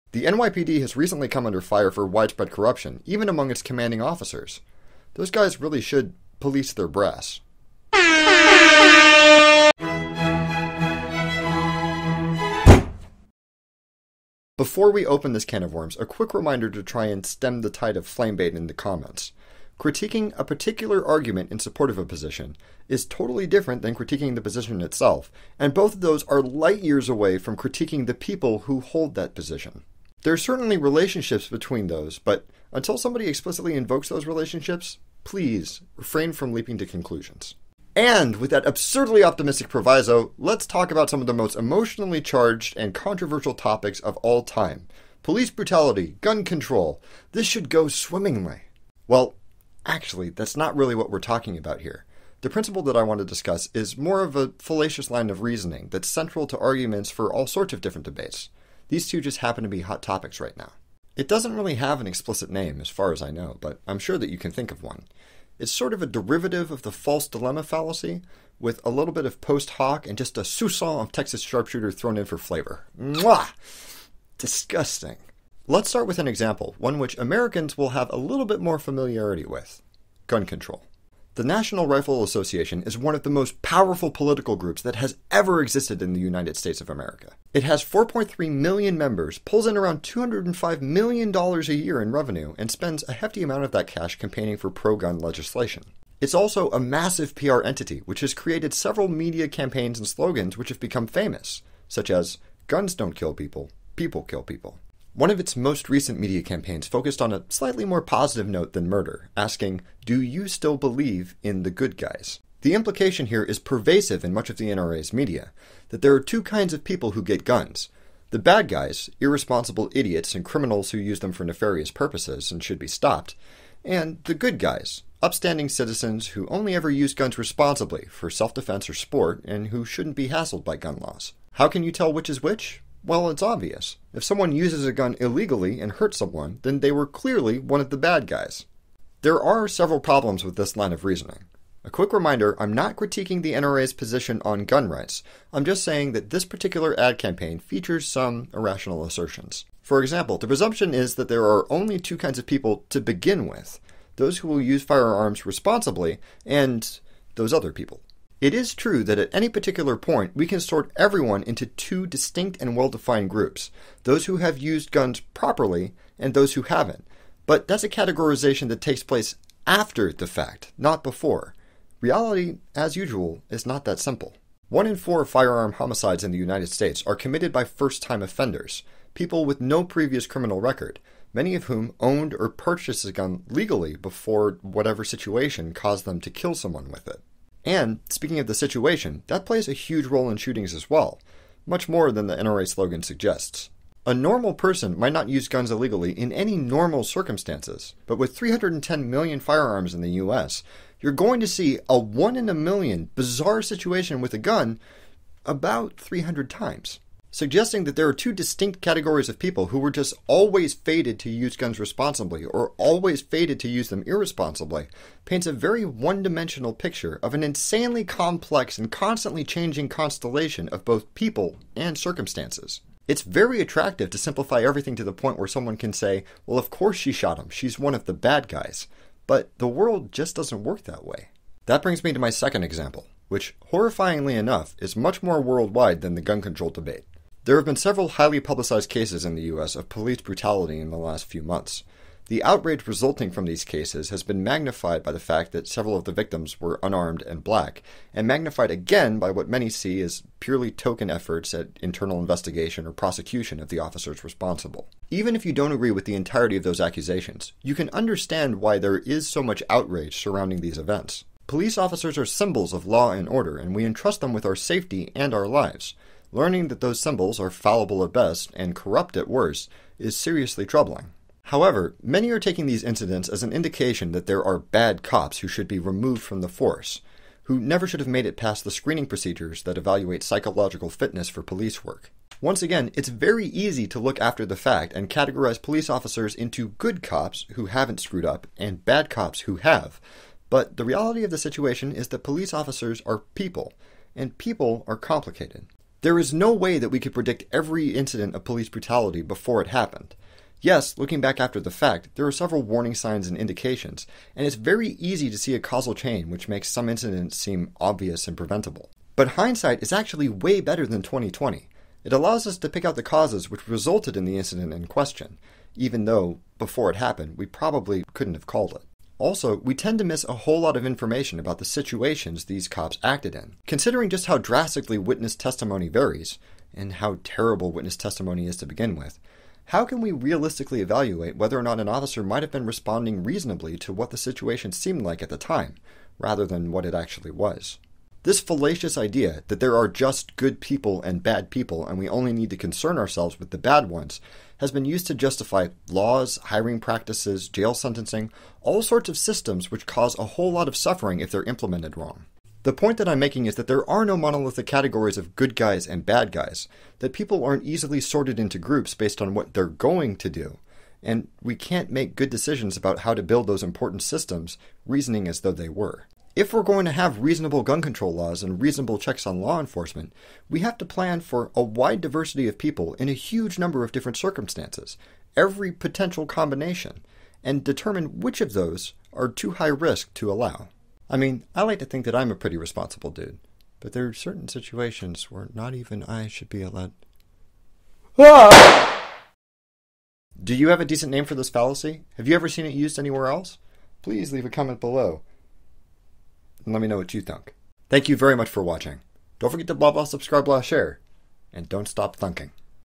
67. Good Guys, Bad Guys, & Fallacies | THUNK
What do gun control & police brutality have in common? Some arguments for both contain the same logical fallacy.

An oddly appropriate article about "good cops" and "bad cops," as well as firearm ownership